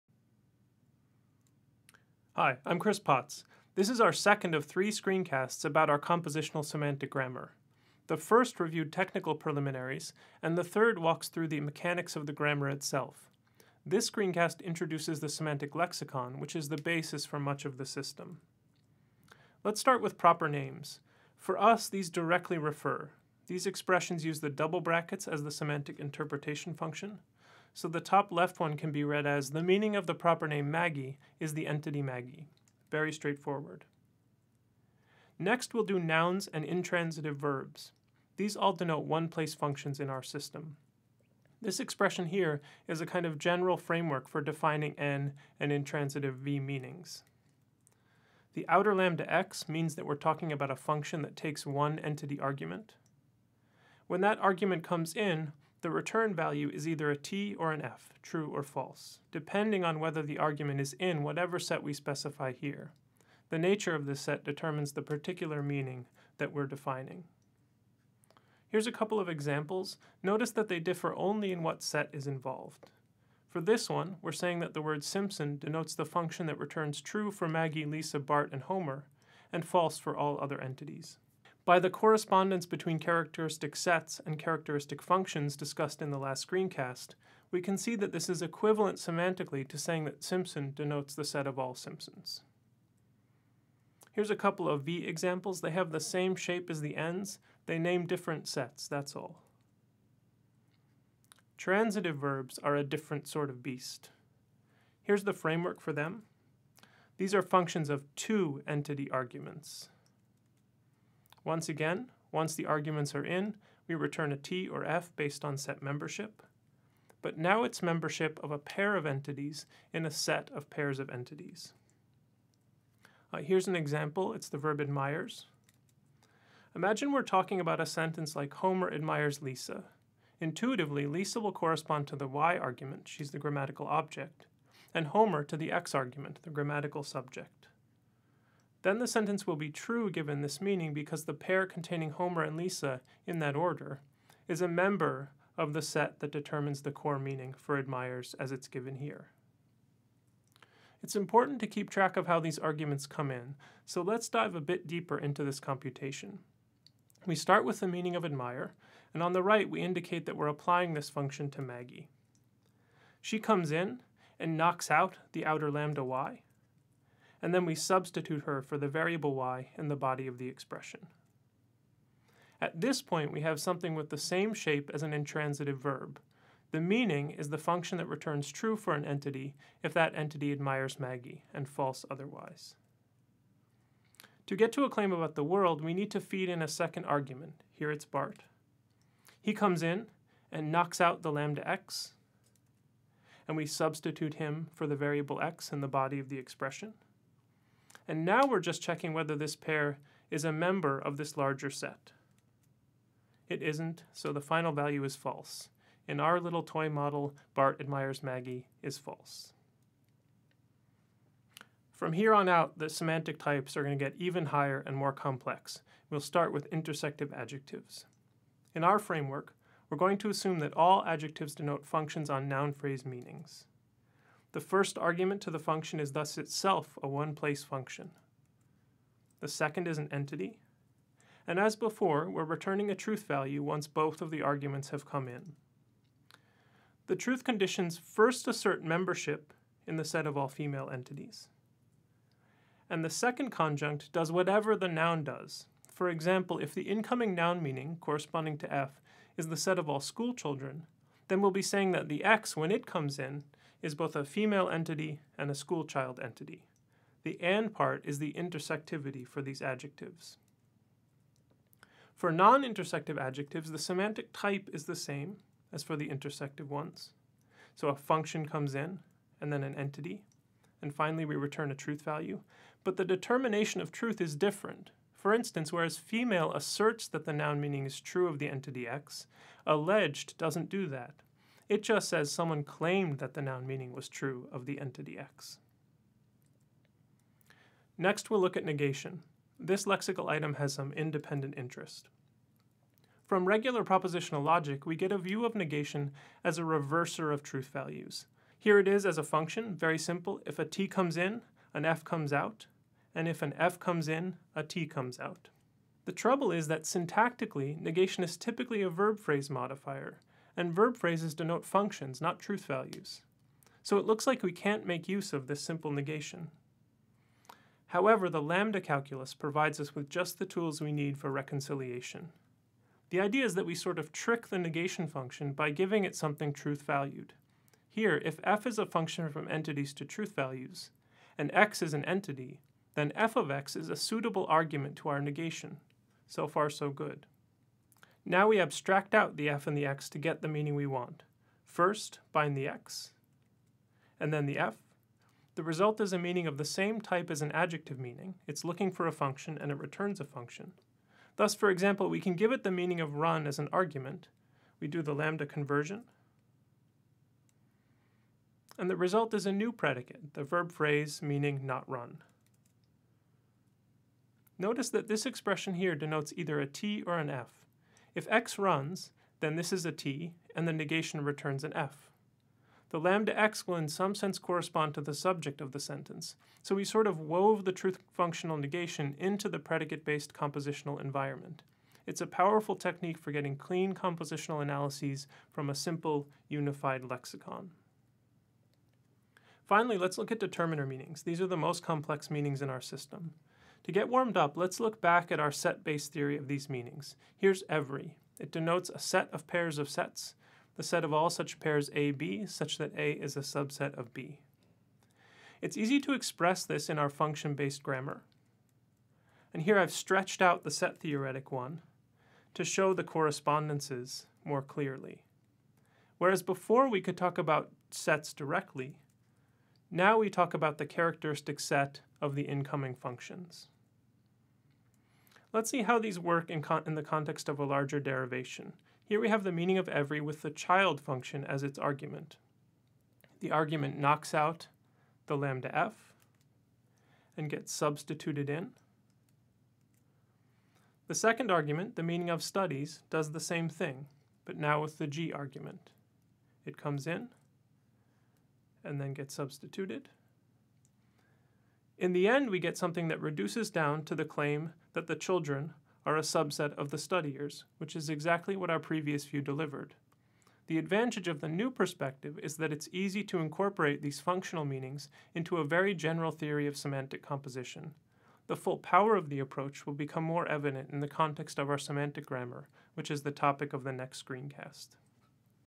Linguist 130a - Semantic composition 2: Lexicon
A semantic lexicon in which meanings denote functions that we identify using an informal version of the typed lambda calculus.